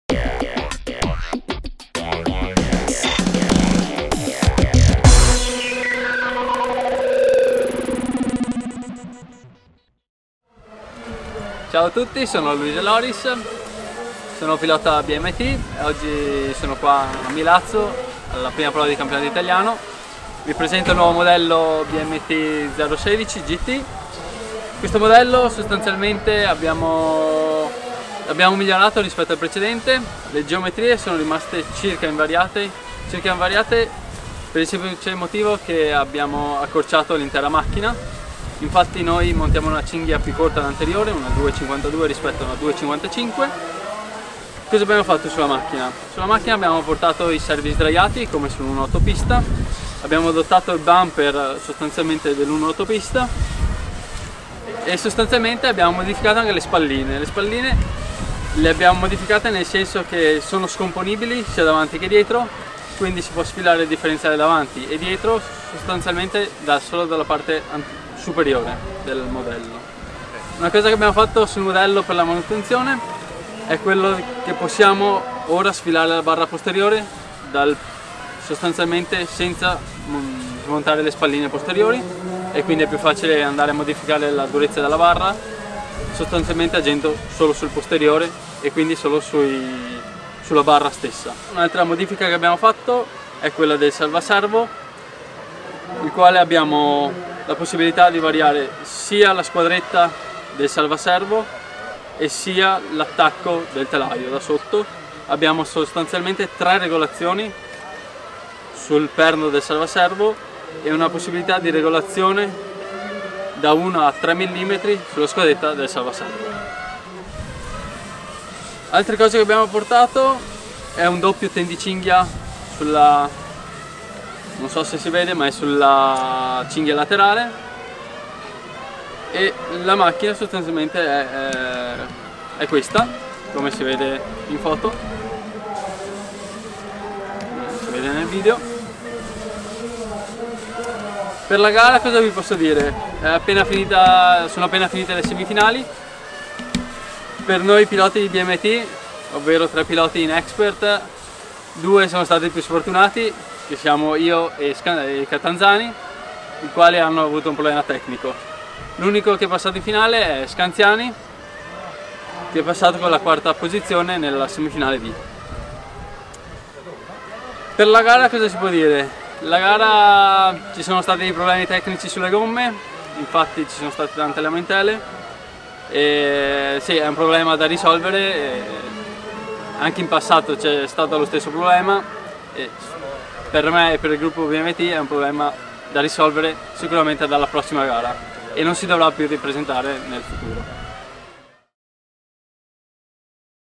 C.I. GT - Intervista a Loris Luise - BMT 016 GT
Live Report da Milazzo per la prima prova di Campionato Italiano A.m.s.c.i. GT.  Intervista a Loris Luise - BMT 016 GT